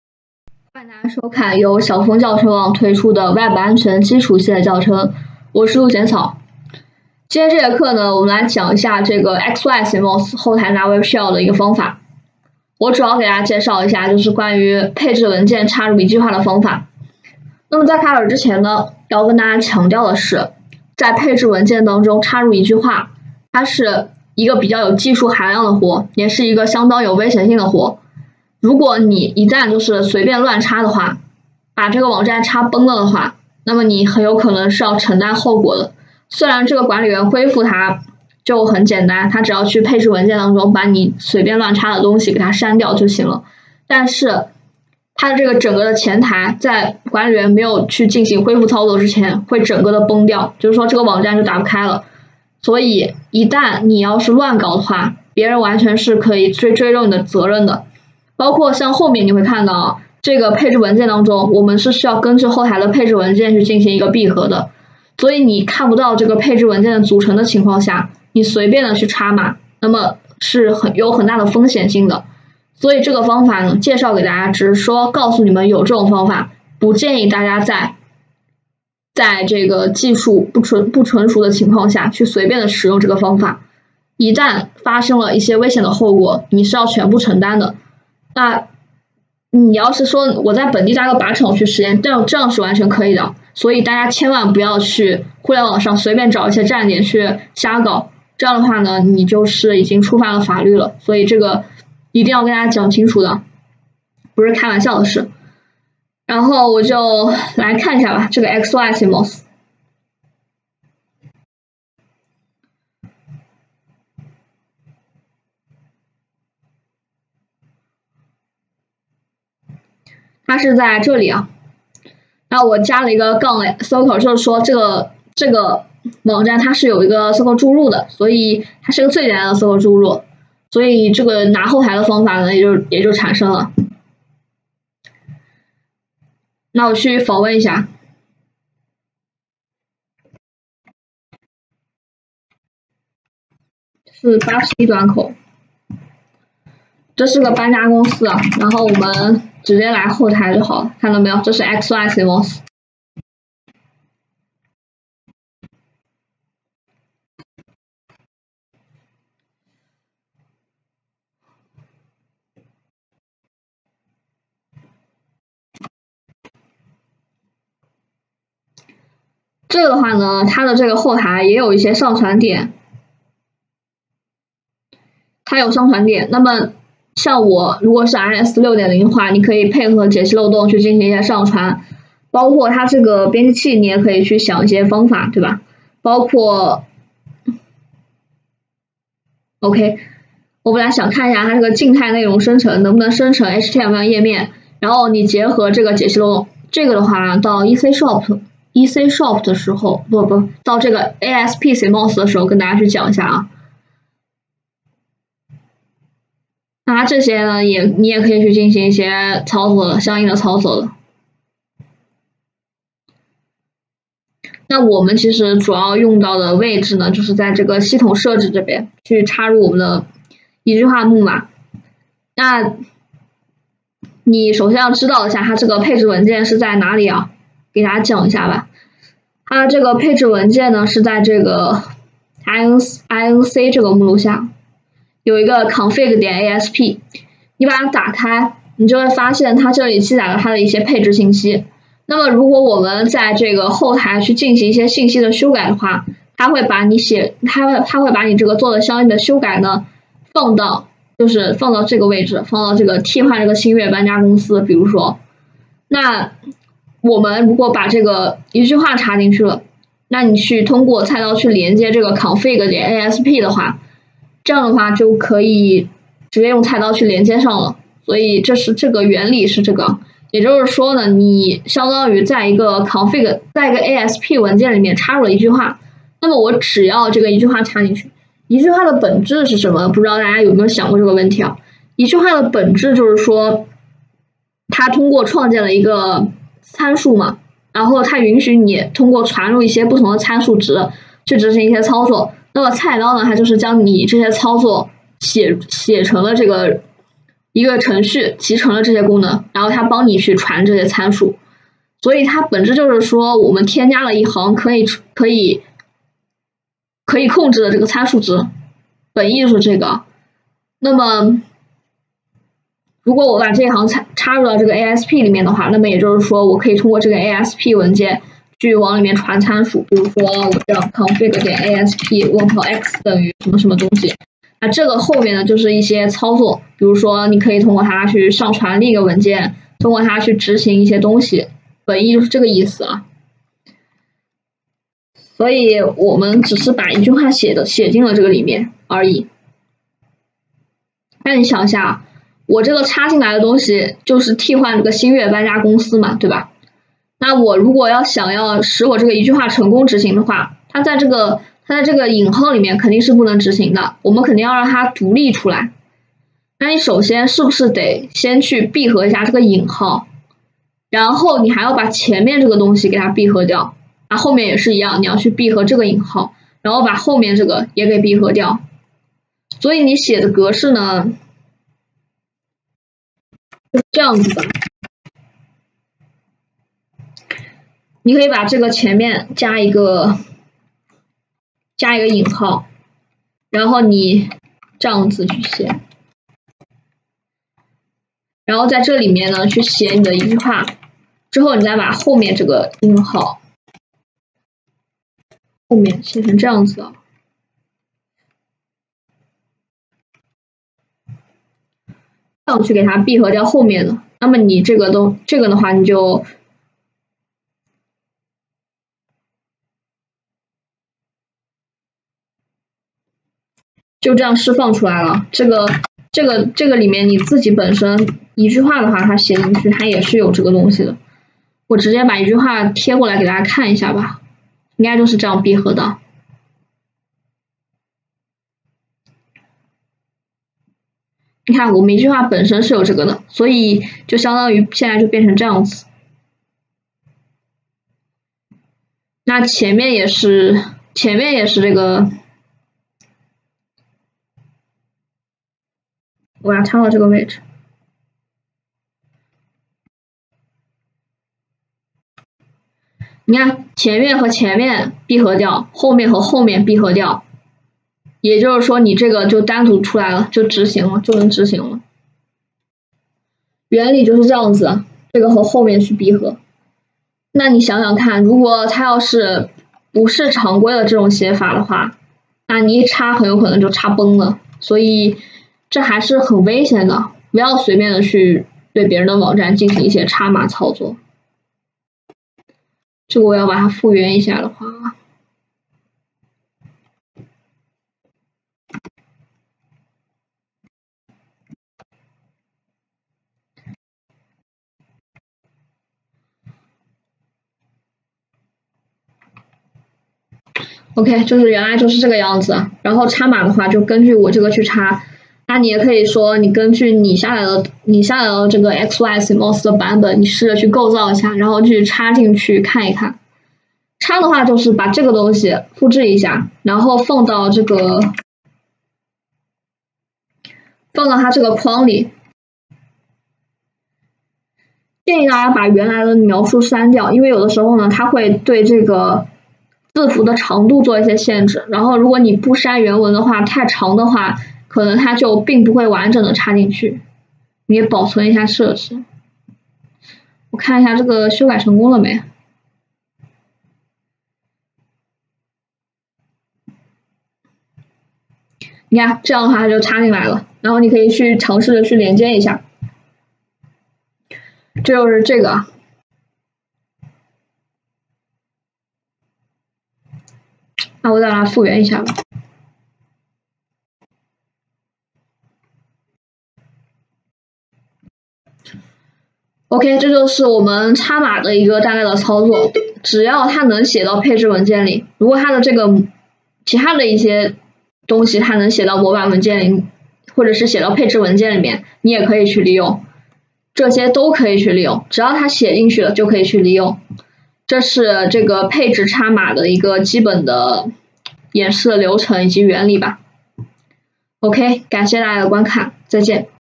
6 xycms
课程简介
本课程通过讲解常见的cms后台如何获取webshell，帮助学员了解学会利用后台获取webshell的常见方法和思路。